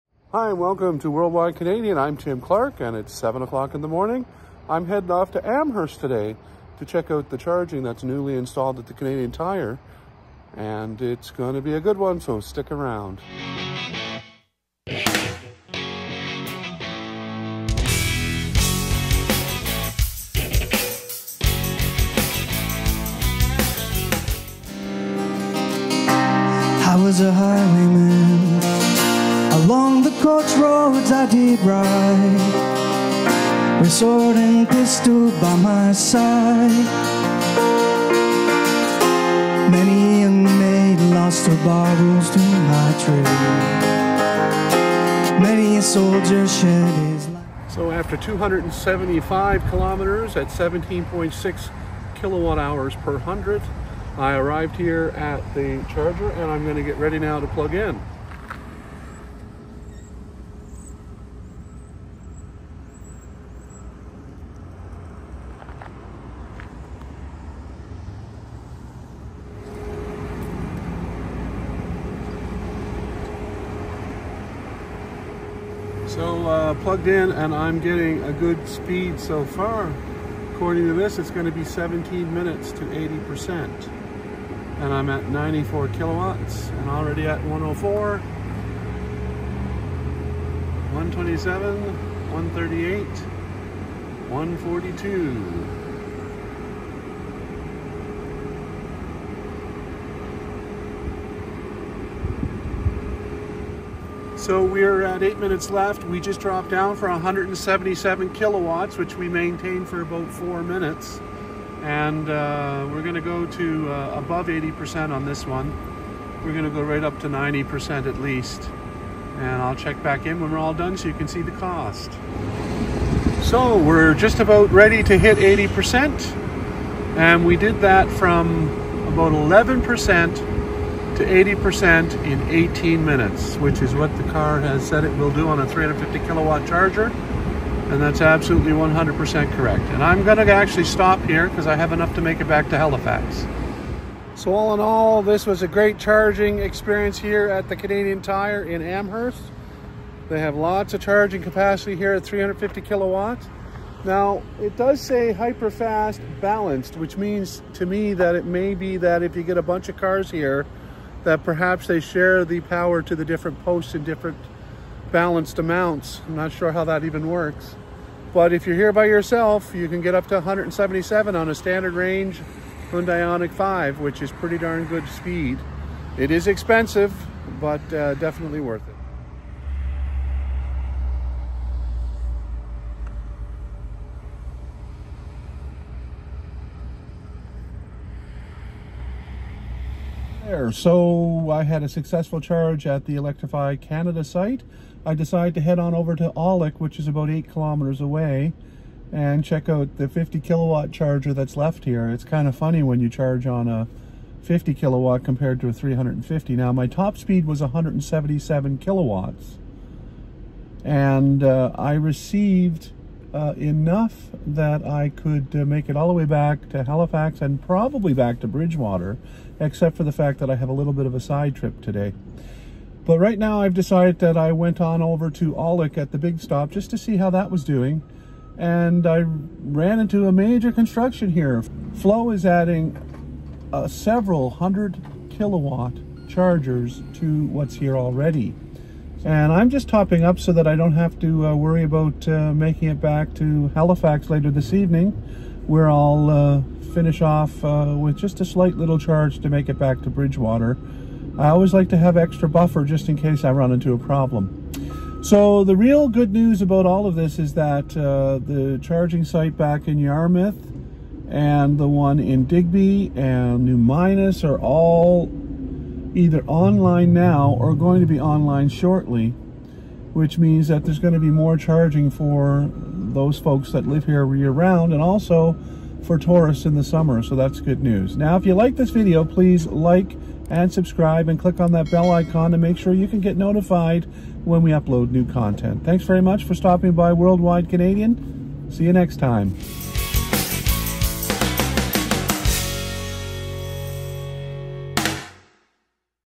World Wide Canadian Electrify Canada in Amherst NS
Lots of visitors and residents in Nova Scotia have had little options to select for charging and it is getting better and this site in Amherst is a perfect example with 6 Ultra Fast Chargers which will alleviate some of the congestion in places like Masstown with only one 50KW charger. If visitors come across the NB border charging in Amherst will allow them to make it to the chargers that are in Halifax. They can also likely make the additional Flo chargers in Stellerton and Monastery. There are also projects started in Lower Sackville at the Canadian Tire there. Another Electrify Canada site will offer additional options for travellers. With the announcement of GM now being able to use the Tesla chargers at some places like Charlottetown and New Brunswick options are increasing.